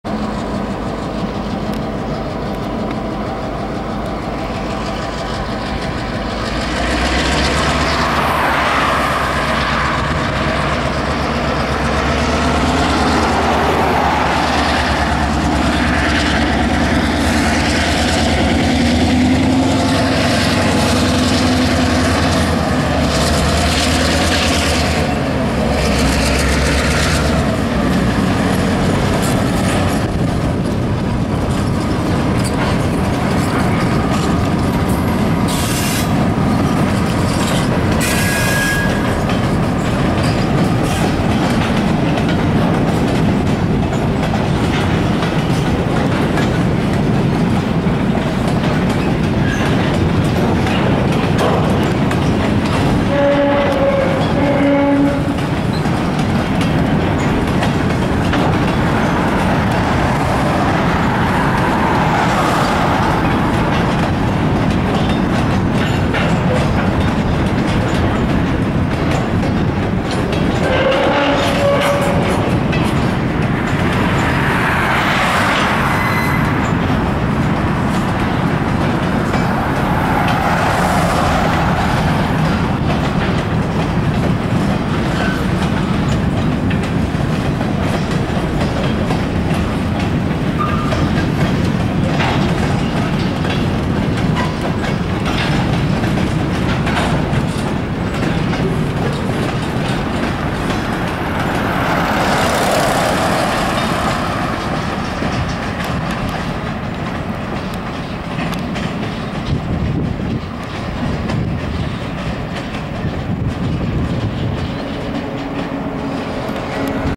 ELS TRAIN!!! ELS400 next to mead paper in Escanaba/ Danford MI 6-12-23
sub please

11:45AM EDT